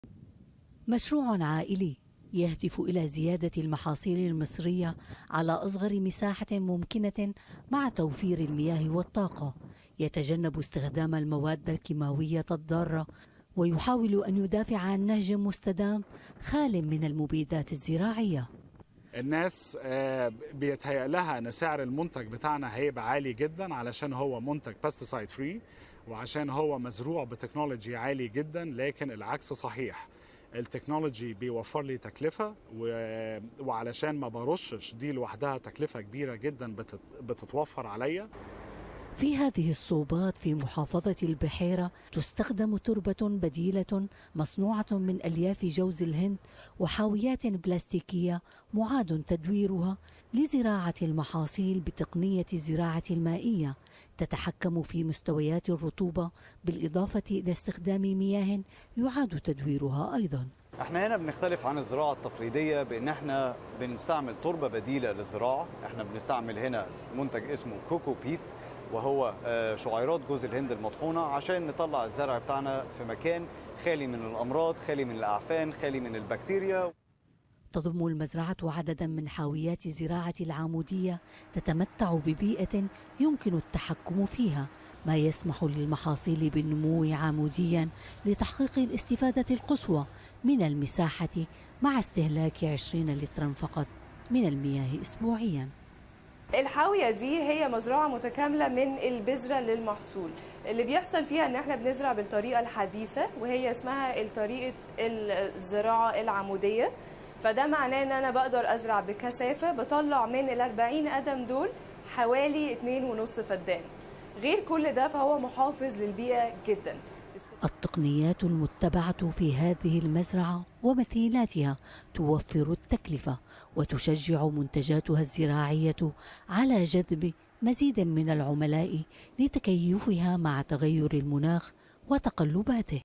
مزارع توليمة في مصر تواجه التغير المناخي
يستخدم أصحاب مزارع في محافظة البحيرة بمصر صوبات زراعية آلية وخاضعة للمراقبة لتتبع حالة المحاصيل وجعلها تتكيف مع التغيرات المناخية.
تقرير: بهية مارديني